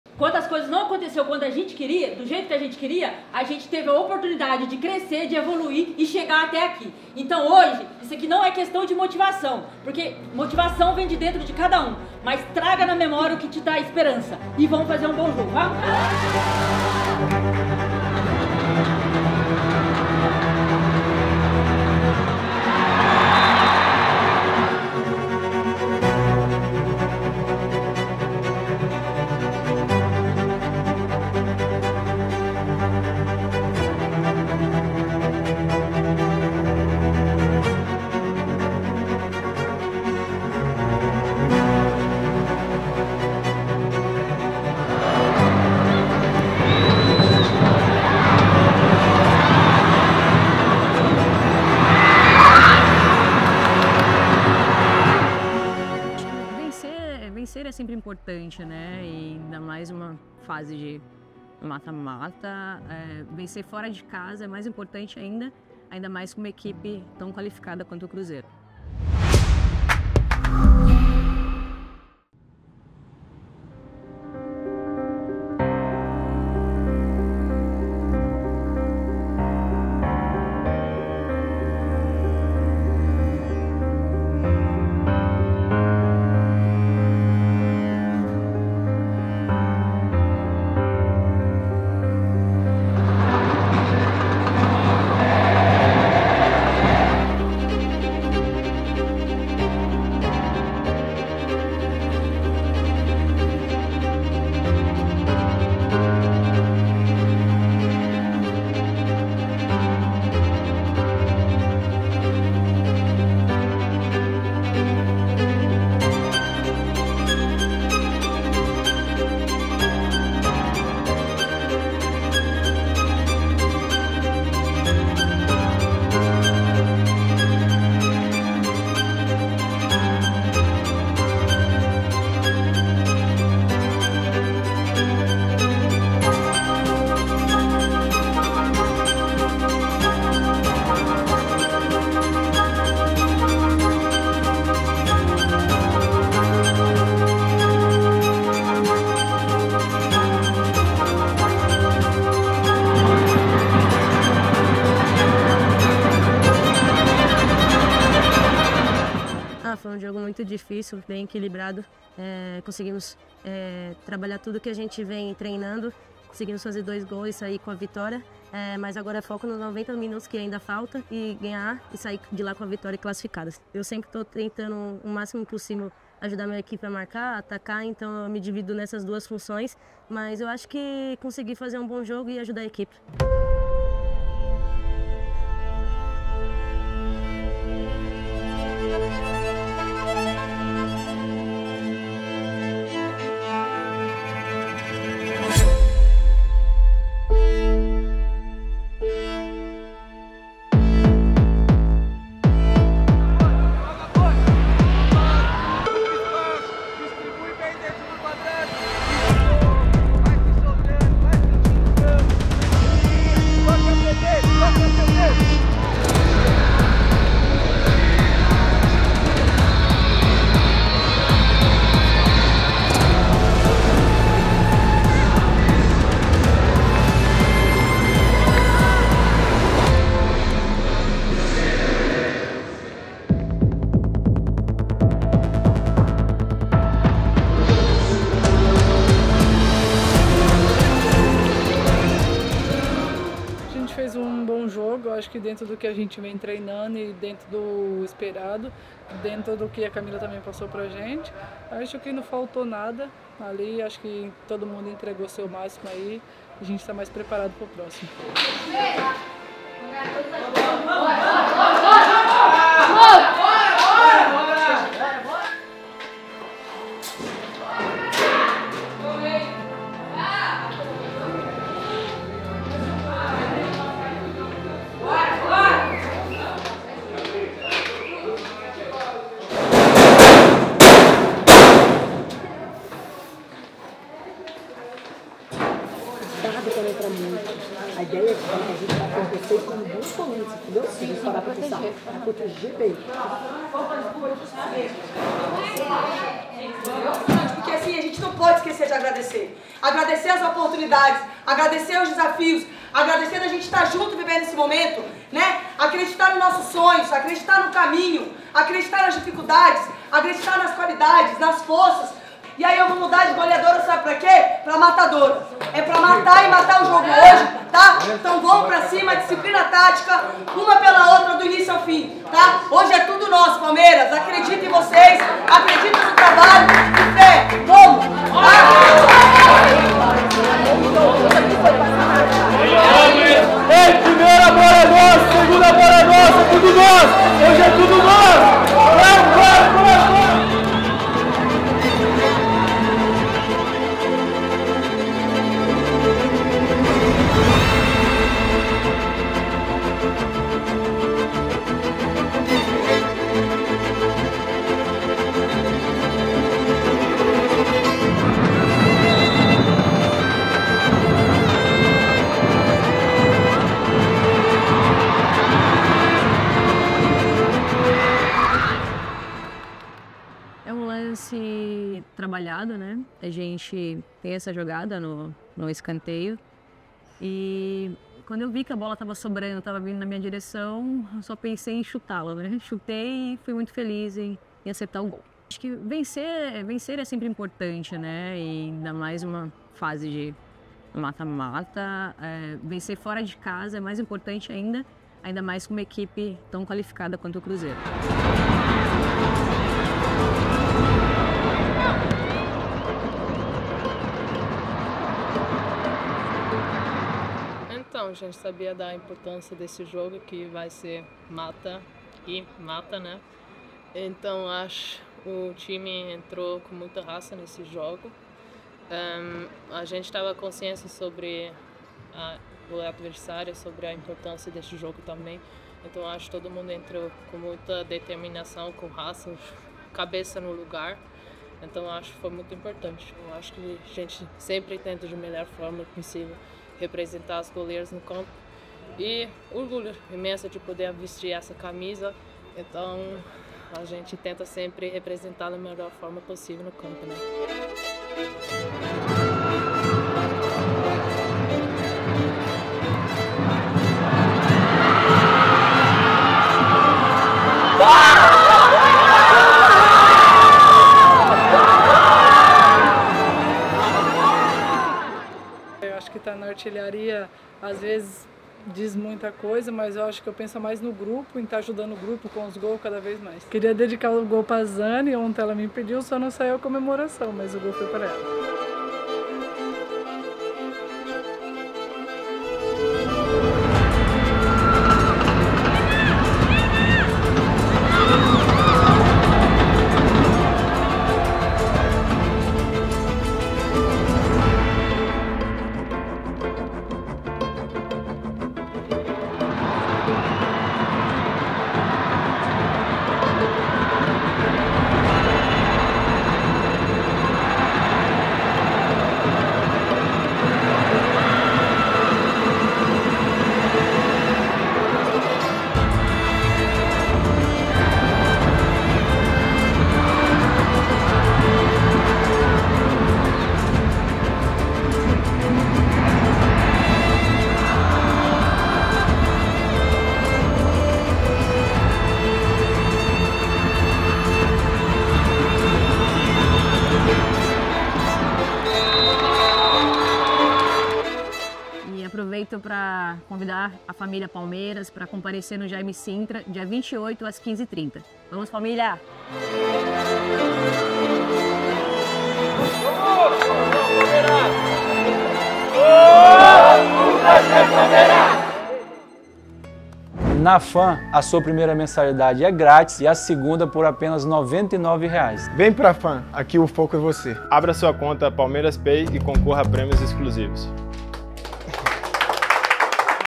BASTIDORES | CRUZEIRO 1 X 2 PALMEIRAS | BRASILEIRO FEMININO 2024
O Palmeiras venceu o Cruzeiro por 2 a 1, neste domingo (25), pelo jogo de ida das quartas de final do Brasileiro Feminino, na Arena Independência, em Belo Horizonte-MG. Os gols foram marcados por Juliete e Amanda Gutierres.
Conteúdo Powered by @CanonBR
Concorra a mais de 600 prêmios durante o ano no Palmeiras Pay Premiado.